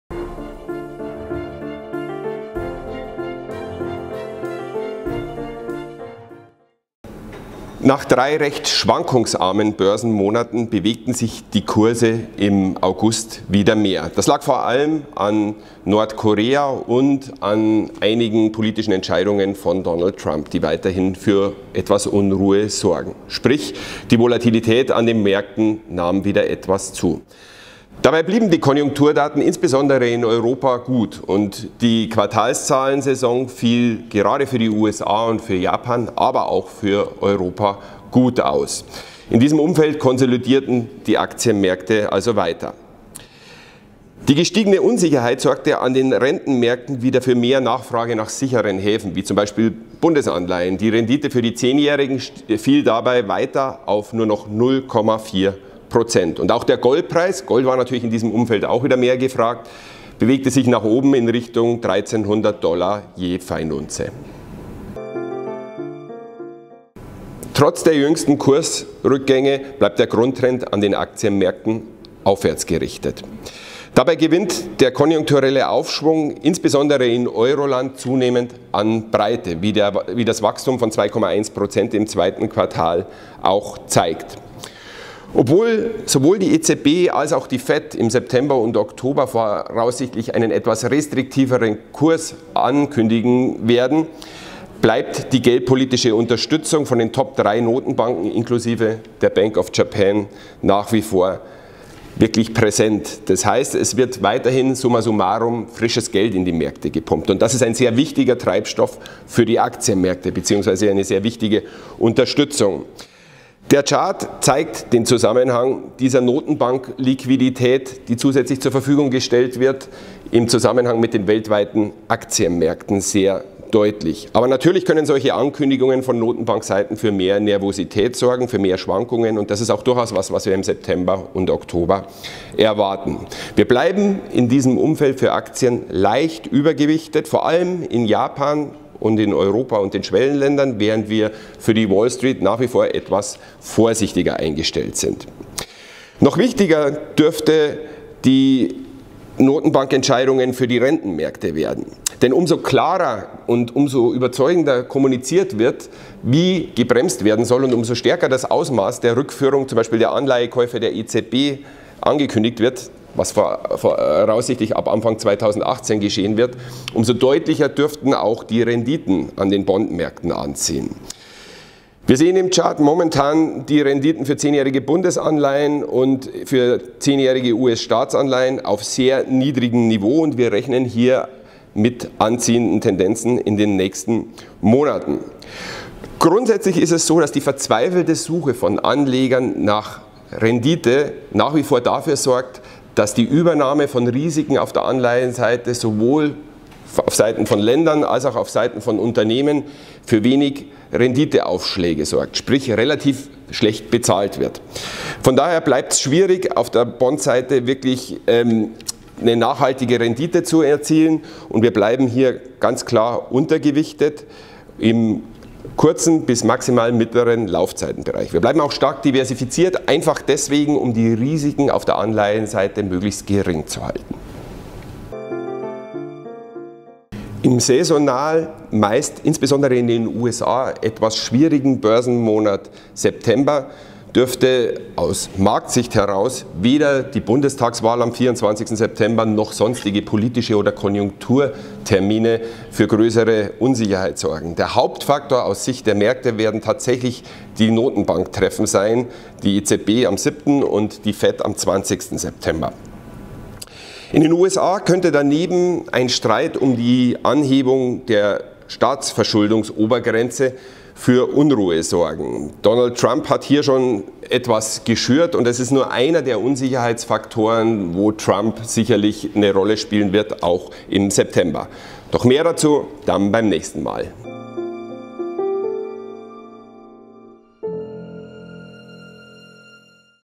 Märkte & Trends - September 2017
Marktausblick für September 2017 mit Chefstratege Robert Greil